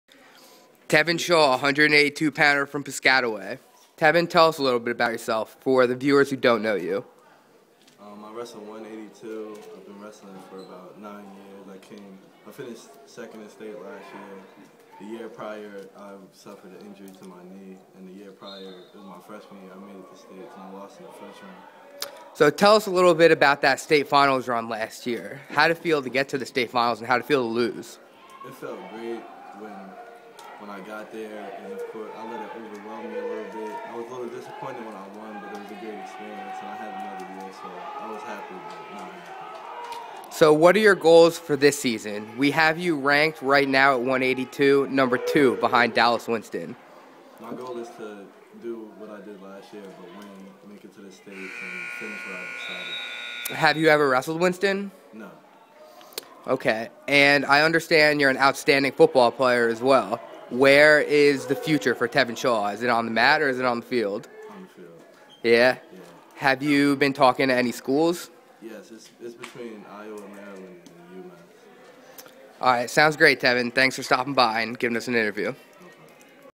Tevin Shaw Interview.MOV
Tevin Shaw, 182lber from Piscatiway, talks about his aspirations for 2012 and his future beyond the mat.